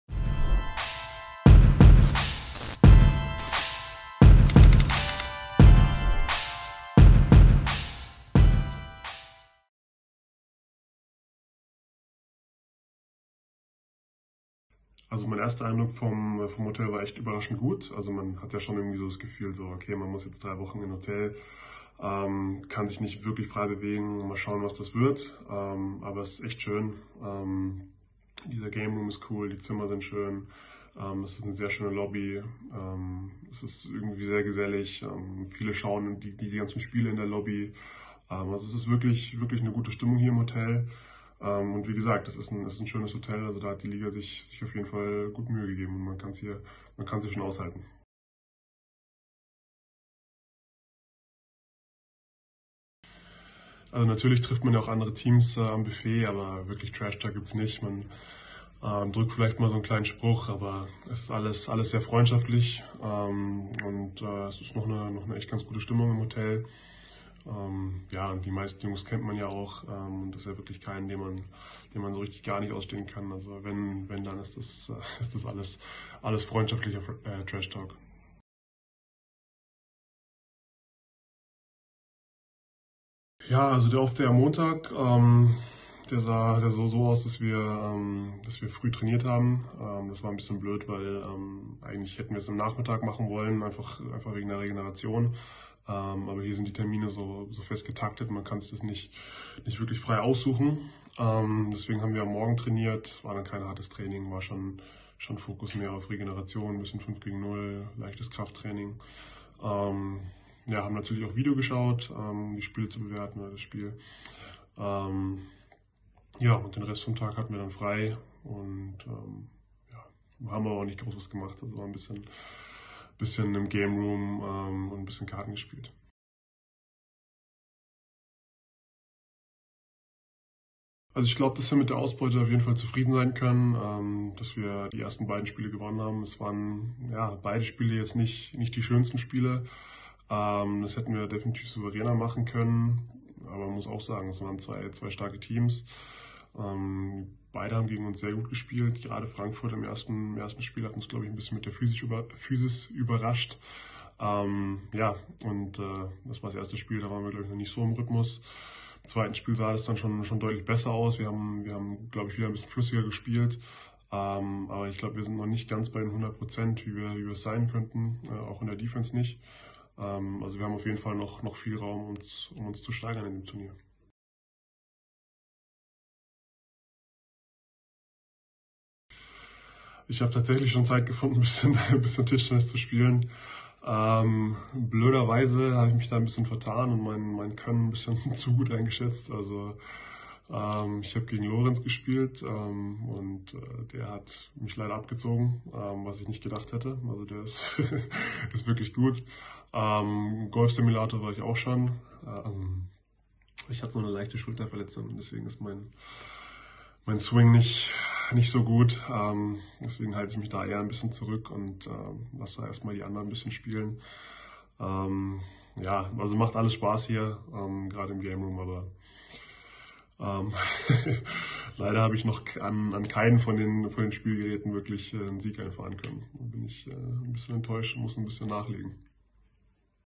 "Wir haben auf jeden Fall noch viel Raum, um uns zu steigern!" | DBB-Center Johannes Thiemann
Nach zwei Siegen zum Auftakt in das BBL-Finalturnier hat uns A-Nationalspieler und Center von ALBA Berlin Johannes Thiemann ein weiteres Update aus München gegeben: Der DBB-Big Man bewertet die ersten beiden Spiele seiner Albatrosse, wieso das Team noch Luft nach oben hat und berichtet vom Alltag im Hotel.